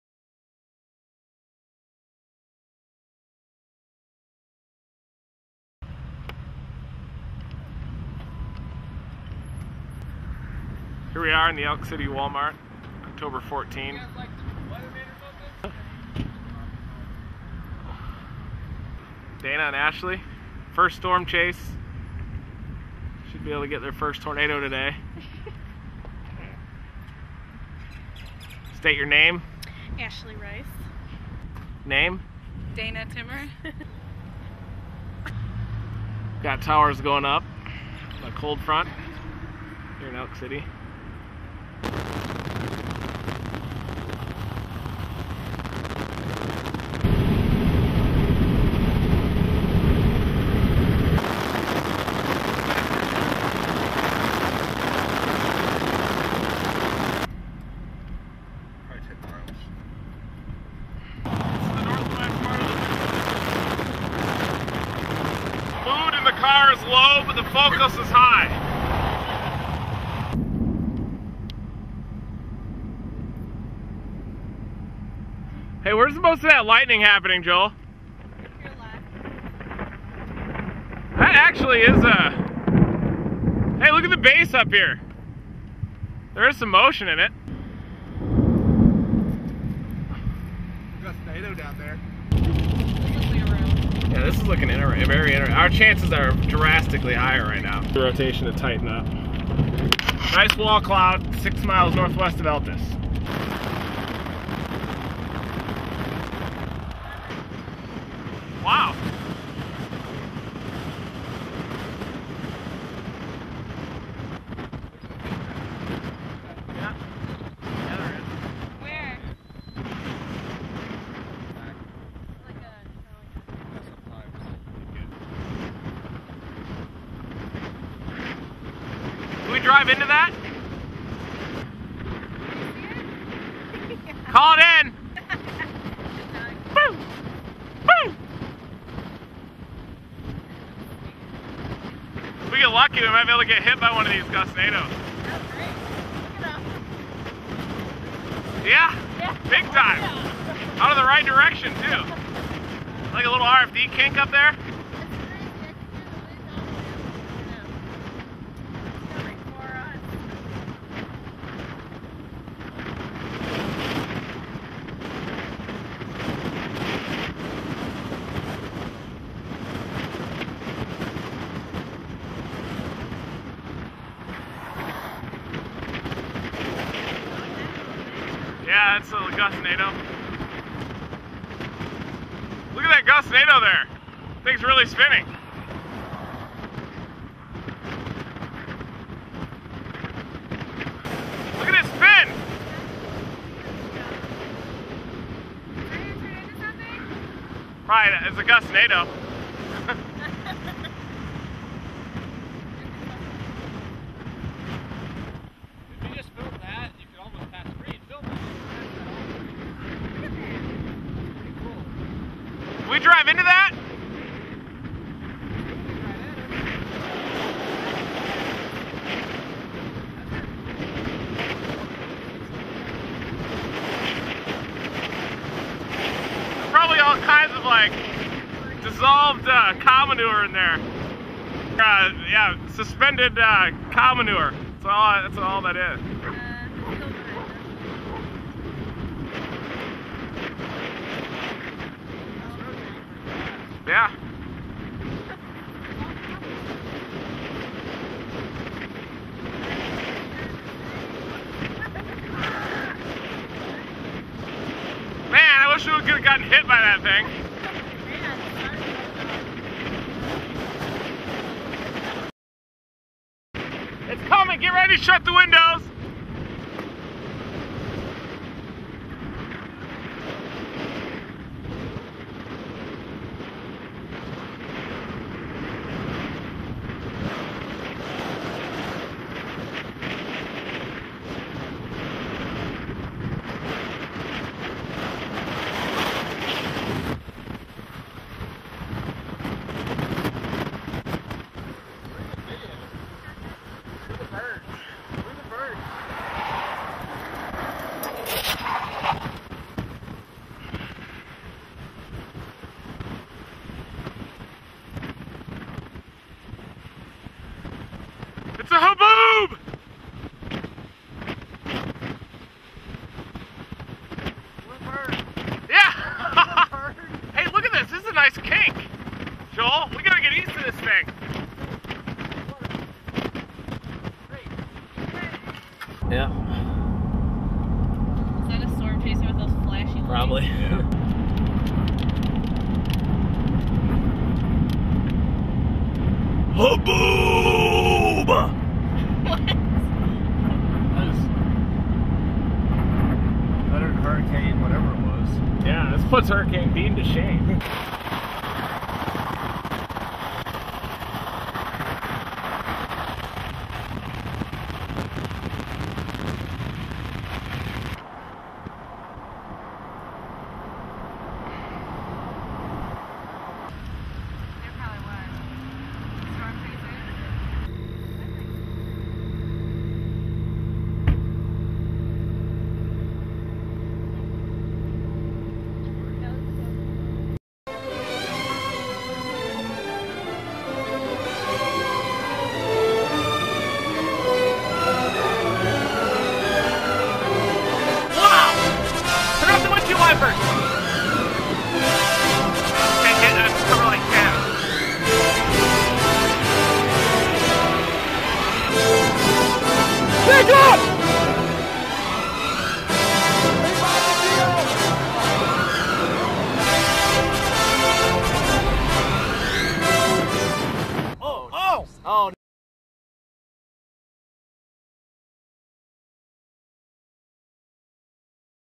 Insane video of Oklahoma gustnadoes from October 14, 2007
TornadoVideos.net storm chasers captured this video of incredible gustnadoes in southwest Oklahoma on October 14, 2007, as a strong cold front surged southward across the Southern Plains.  Gustnadoes are non-supercell tornadoes that often form along outflow boundaries of thunderstorms, and usually consist of a mass of rotating dust.  Check out TornadoVideos.net for insane tornado footage, breaking news, and our live GPS tracker!